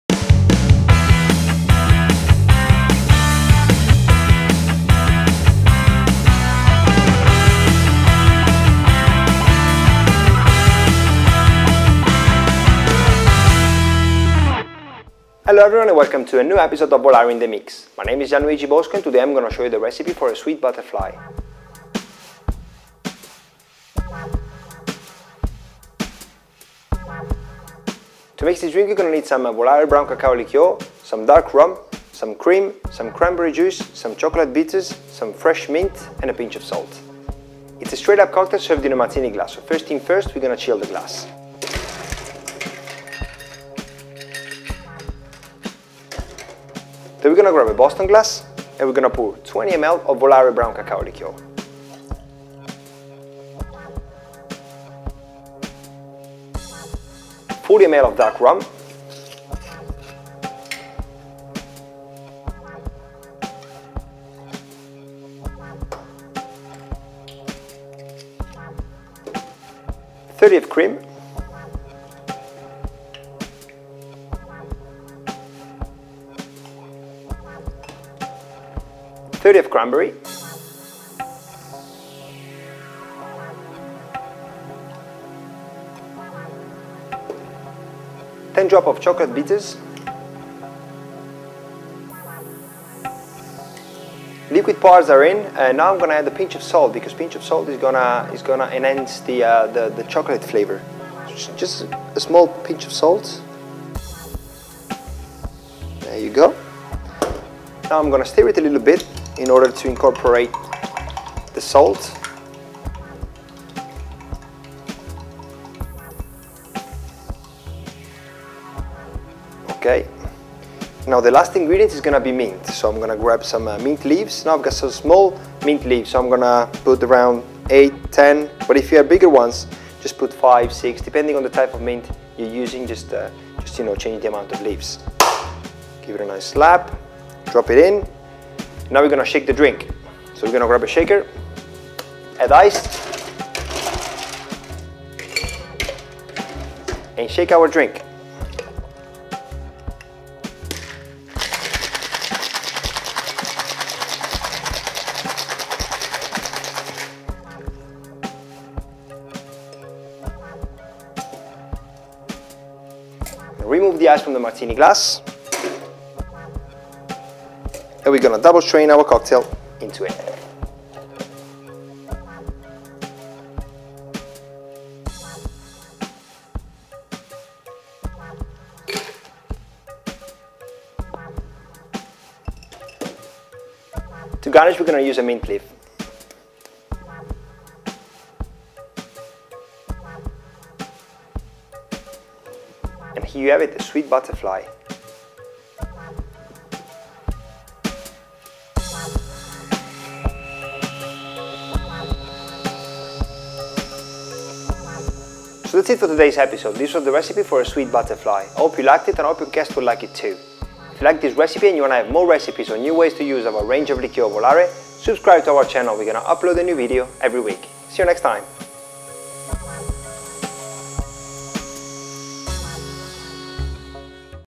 How to mix a Sweet Butterfly - Volare In The Mix episode 67
Gianluigi Bosco, Volare global brand ambassador, will show you how to mix a very interesting after dinner.
Here's the recipe:

-20ml Volare Brown Cacao
-40ml dark rum
-30ml cranberry juice
-30ml cream
-10 drops chocolate bitters
-6/8 mint leaves